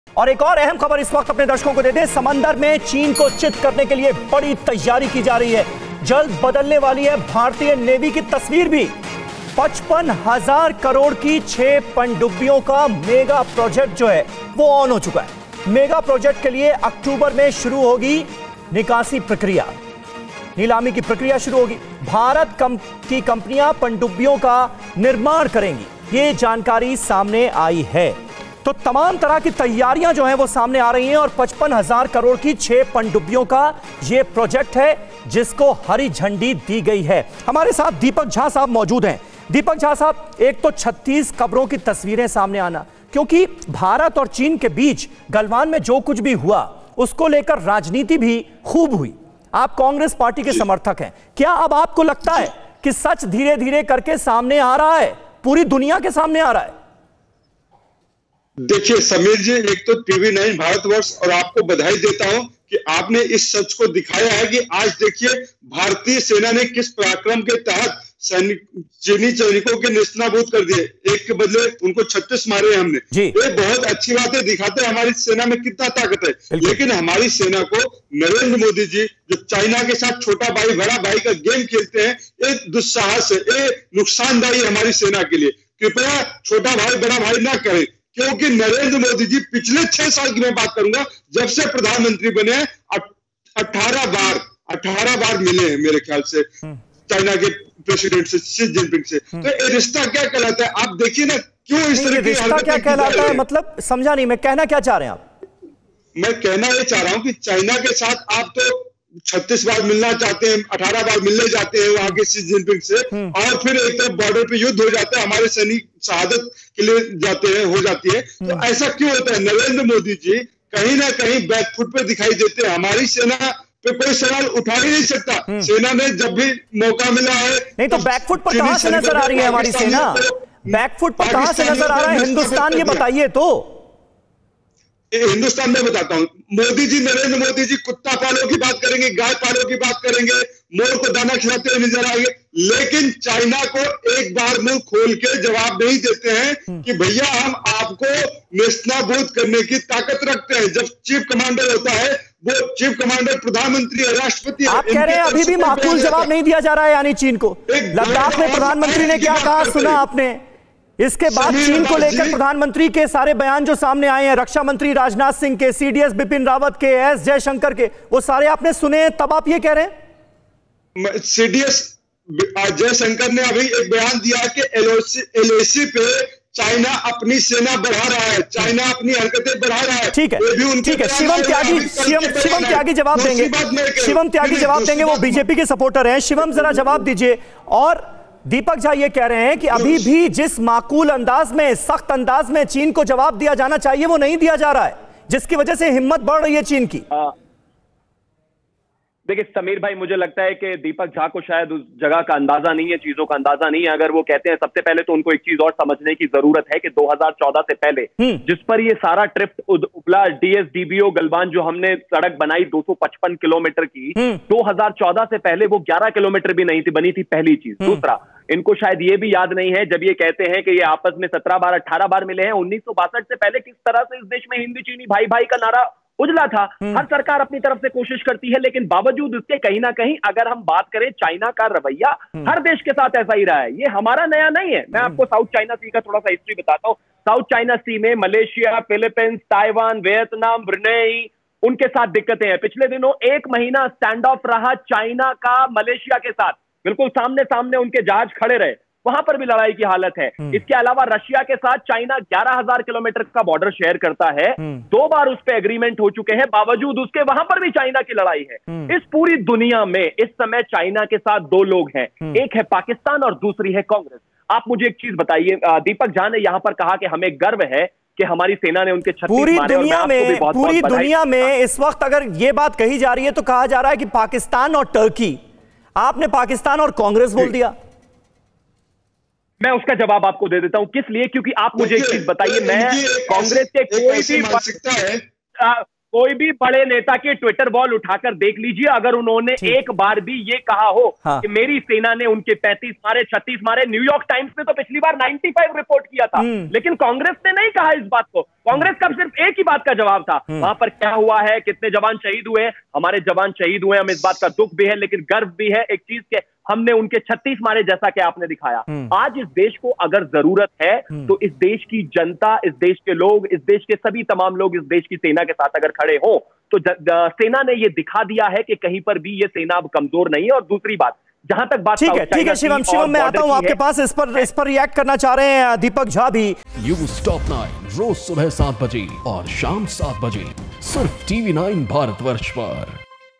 सेना के शौर्य के सामने देश नतमस्तक, PM की चुप्पी पर विरोधियों के सवाल
Indian Army का Galwan Valley में शौर्य देखकर दुनिया नतमस्तक है. मामले को लेकर मगर राजनीति भी खूब गरम है. विरोधी मुद्दे को लेकर PM की खामोशी पर सवाल उठा रहे हैं.